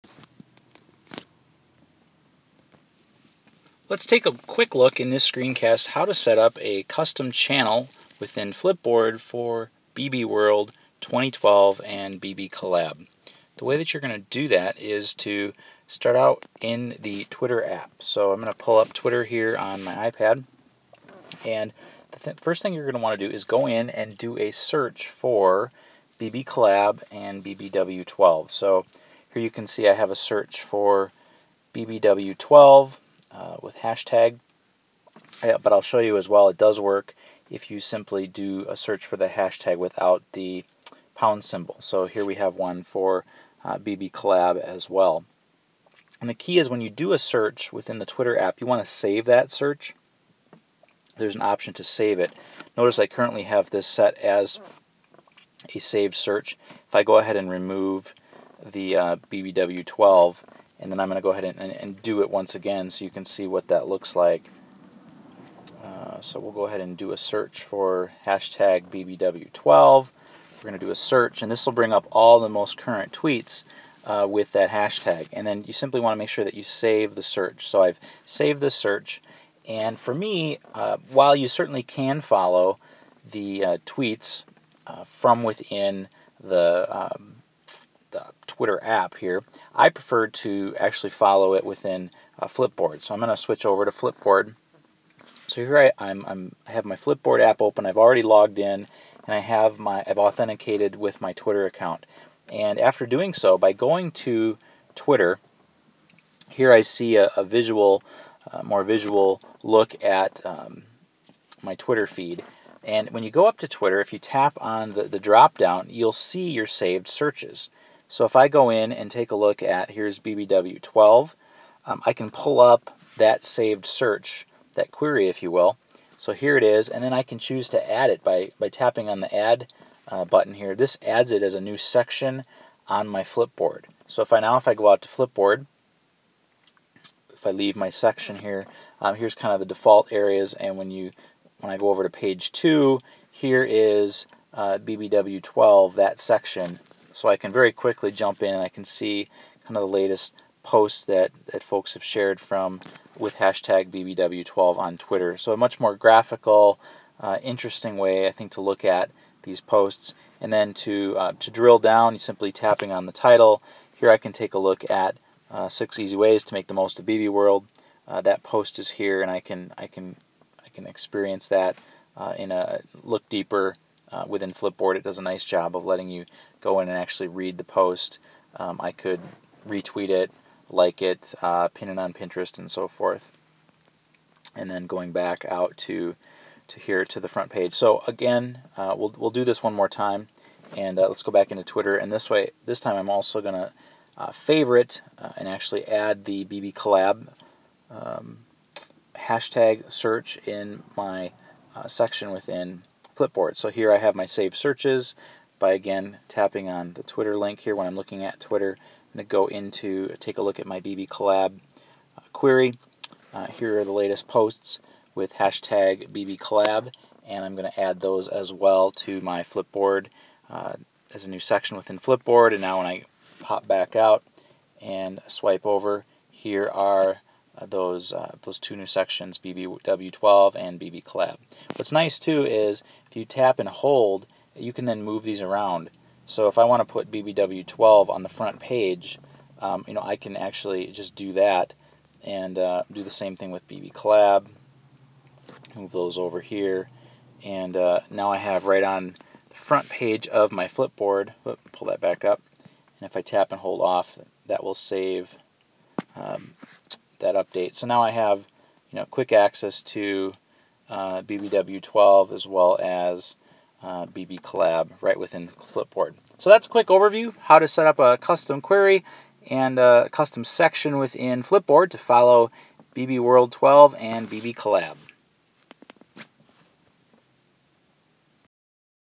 How to Setup Custom Flipboard Section for BbWorld
steps for how to setup a custom section in Flipboard for following saved Twitter searches BbW12 and BbCollab